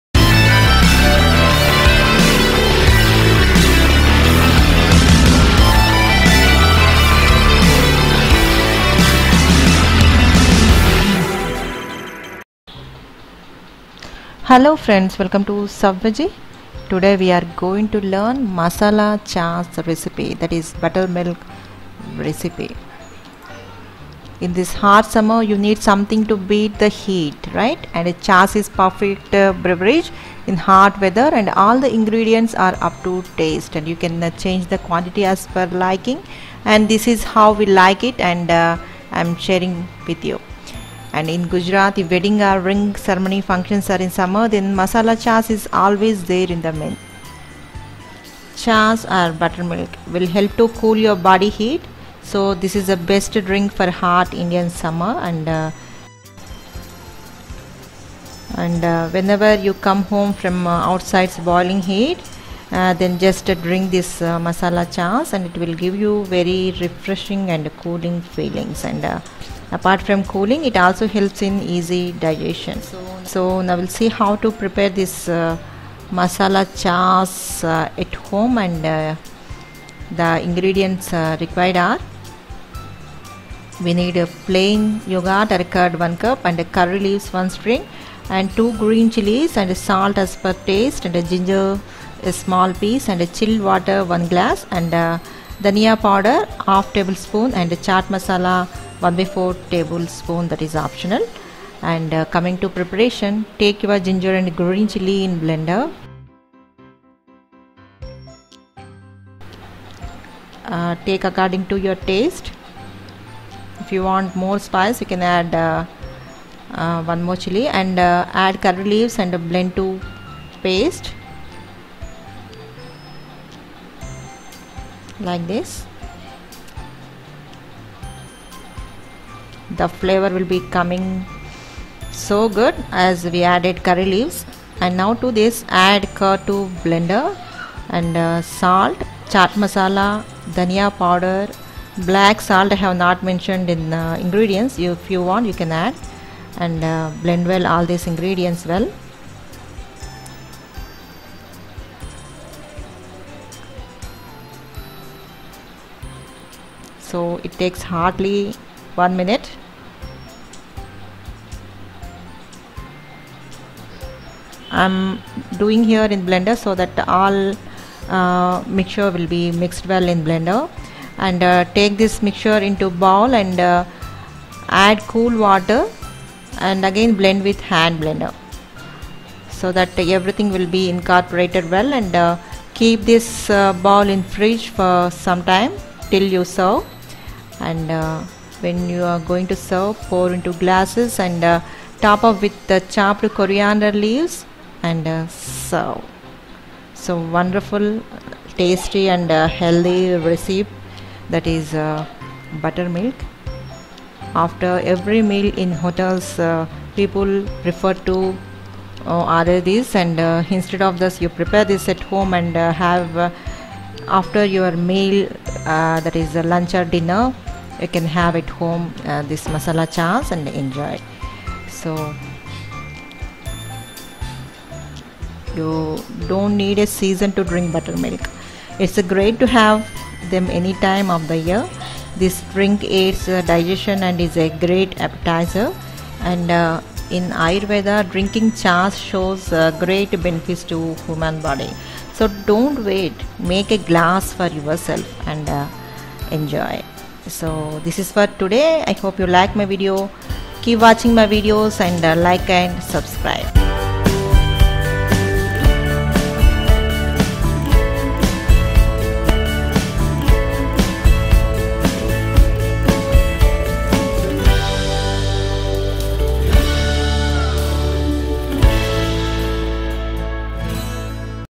MASALA CHAAS RECIPE
super coolent recipe for summer, easy to prepare and good for health,,particularly ginger and curry leaves help in digestion...just have a glass of butter milk after your every meal and be healthy...its easy to prepare and not only in summer in every season its favorite to all
ll...so just have a glance to know the recipe and prepare with in no time and enjoy flavored buttermilk in your meal...